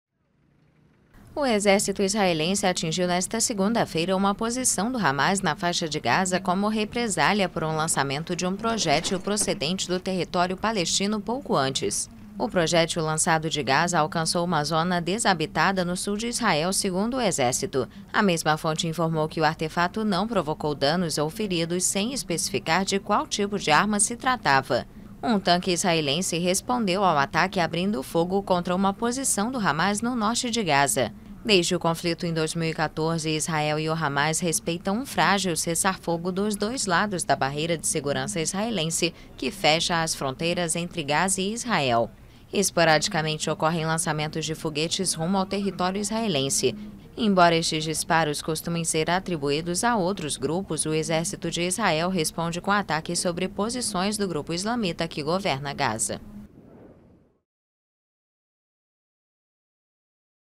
Israel atinge posição do Hamas
O Exército de Israel atingiu uma posição do Hamas na Faixa de Gaza. A ação ocorre pouco depois que um projétil foi lançado do território palestino.